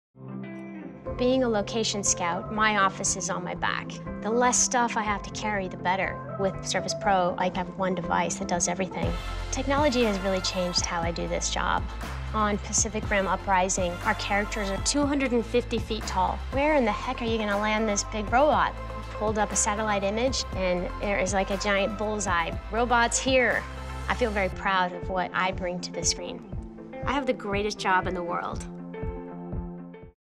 Surface Pro and Leann Emmert –Scouting locations for Pacific Rim Uprising
Leann Emmert travels the world looking for the perfect places to shoot films. And when your main characters are 250 ft. robots, that’s no easy feat. With the new Surface Pro, she can edit her drone footage, retouch her photos and create presentations, almost everywhere she goes. Makes working on the go a whole lot easier.